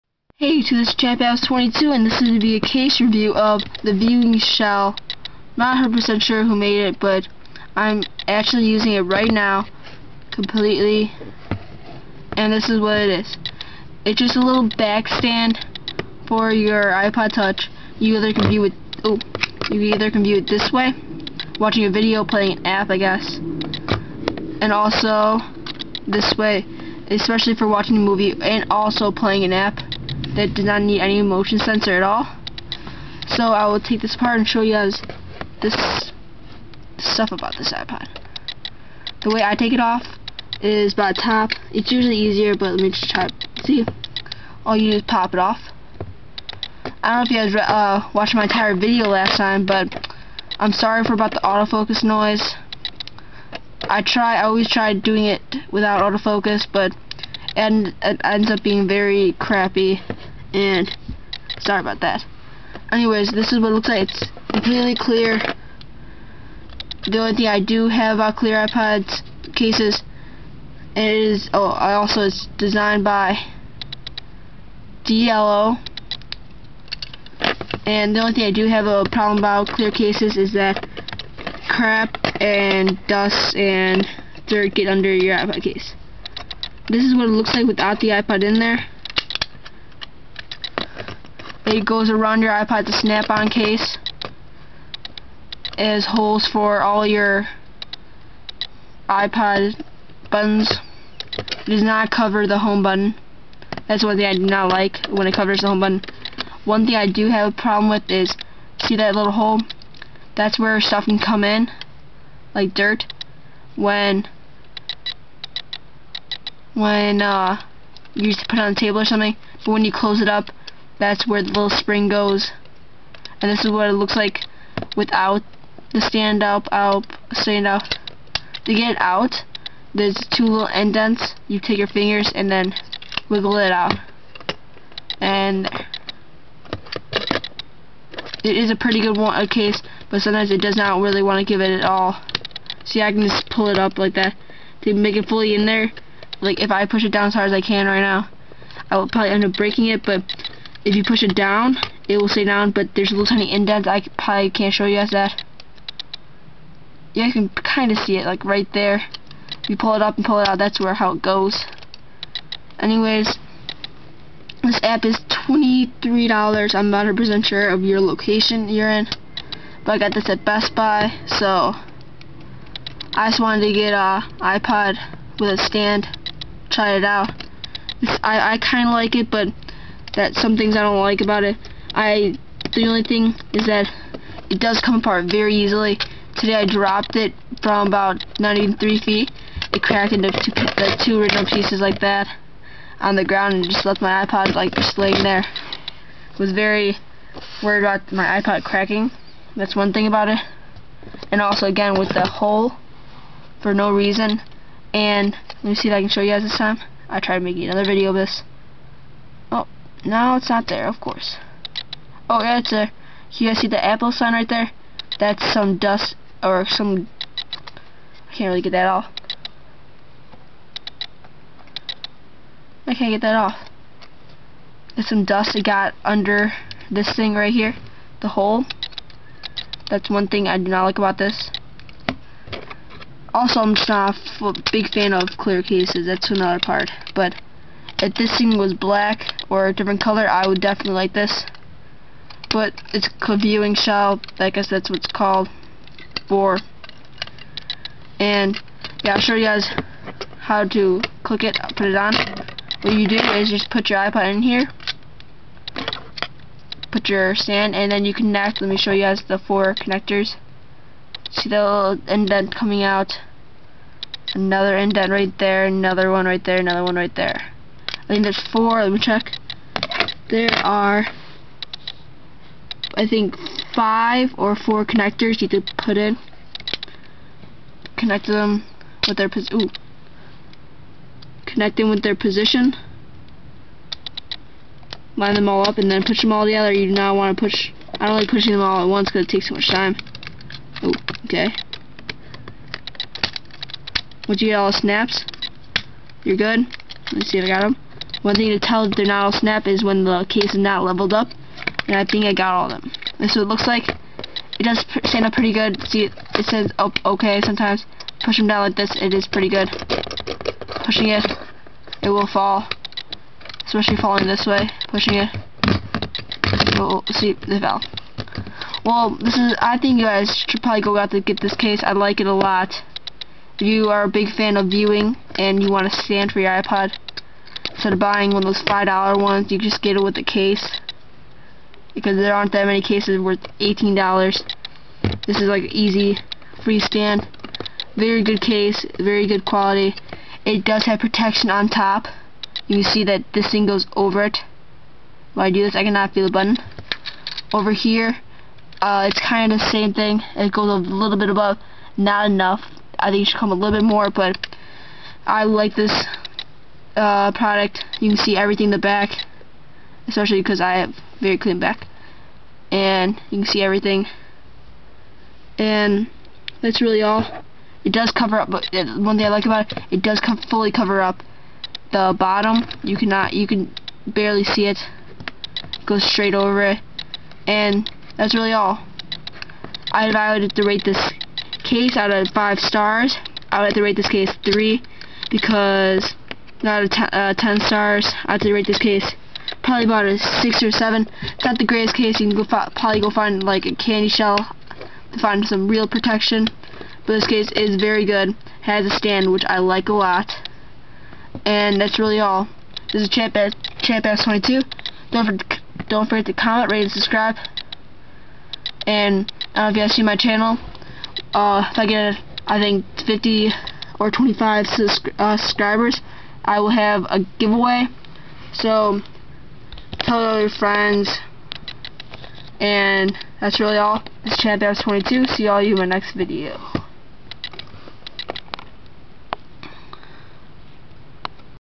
Case Review of The DLO VdieoShell
This case is very nice for watch videos and  to play app too.
This product does work on the ipod touch 2g and 3g
price- $20